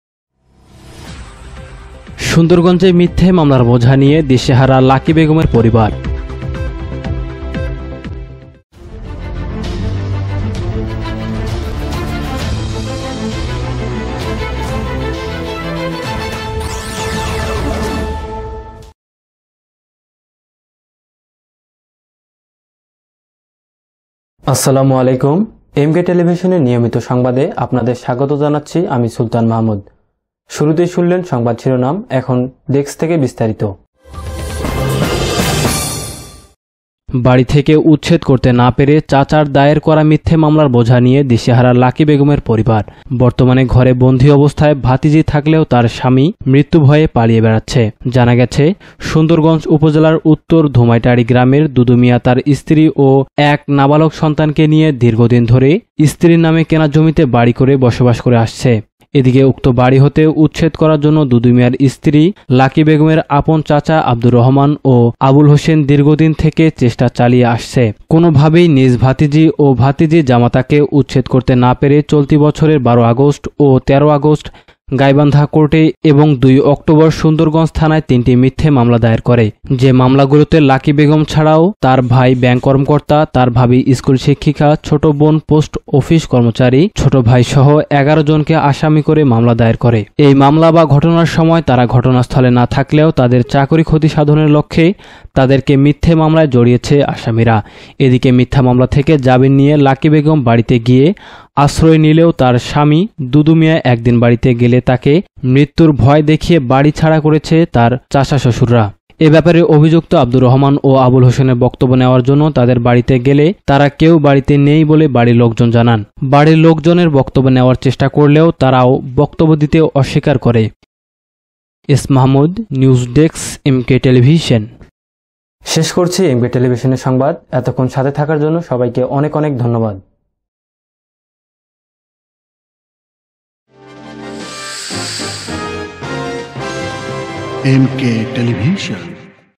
সুন্দরগঞ্জে মিথ্যা মামলার বোঝা নিয়ে দিশেহারা লাকী বেগমের পরিবার । MK Tv News
সুন্দরগঞ্জে মিথ্যা মামলার বোঝা নিয়ে দিশেহারা লাকী বেগমের পরিবার

খালেদ হোসেন ,গাইবান্ধা প্রতিনিধি:

 বাড়ি ঘর থেকে উচ্ছেদ করতে না পেরে চাচার দায়ের করা মিথ্যা মামলার বোঝা নিয়ে দিশেহারা লাকী বেগমের পরিবার। বর্তমানে ঘরে বন্দি অবস্থায় ভাতিজি থাকলেও তার স্বামী মৃত্যু ভয়ে পালিয়ে বেড়াচ্ছে ।

 জানাগেছে, সুন্দরগঞ্জ উপজেলার উত্তর ধুমাইটারি গ্রামের দুদু মিয়া তার স্ত্রী ও এক নাবালক সন্তানকে নিয়ে দীর্ঘদিন ধরে স্ত্রীর নামে কেনা জমিতে বাড়ি করে বসবাস করে আসছে। এদিকে উক্ত বাড়ি হতে উচ্ছেদ করার জন্য দুদু মিয়ার স্ত্রী লাকি বেগমের আপন চাচা আব্দুর রহমান ও আবুল হোসেন দীর্ঘদিন থেকে চেষ্টা চালিয়ে আসছে। কোন ভাবেই নিজ ভাতিজি ও ভাতিজি জামাতাকে উচ্ছেদ করতে না পেরে চলতি বছরের ১২ আগষ্ট ও ২৩ আগষ্ট গাইবান্ধা কোর্টে এবং ০২ অক্টোবর সুন্দরগঞ্জ থানায় তিনটি মিথ্যা মামলা দায়ের করে। যে মামলা গুলোতে লাকি বেগম ছাড়াও তার ভাই ব্যাংক কর্মকর্তা, তার ভাবি স্কুল শিক্ষীকা, ছোট বোন পোষ্ট অফিস কর্মচারী, ছোট ভাই সহ ১১ জনকে আসামী করে মামলা দায়ের করে।

এই মামলা বা ঘটনার সময় তারা ঘটনাস্থলে না থাকলেও তাদের চাকুরী ক্ষতি সাধনের লক্ষে তাদেরকে মিথ্যা মামলায় জড়িয়েছে আসামীরা। এদিকে মিথ্যা মামলা থেকে জামিন নিয়ে লাকি বেগম বাড়ীতে গিয়ে আশ্রয় নিলেও তার স্বামী দুদু মিয়া একদিন বাড়িতে গেলে তাকে মৃত্যু ভয় দেখিয়ে বাড়ি ছাড়া করেছে তার চাচা শশুররা।

এ ব্যাপারে অভিযুক্ত আব্দুর রহমান ও আবুল হোসেনের বক্তব্য নেওয়ার জন্য তাদের বাড়িতে গেলে তারা কেউ বাড়িতে নেই বলে বাড়ির লোকজন জানান। বাড়ির লোকজনের বক্তব্য নেওয়ার চেষ্টা করলেও তারাও বক্তব্য দিতে অস্বীকার করে।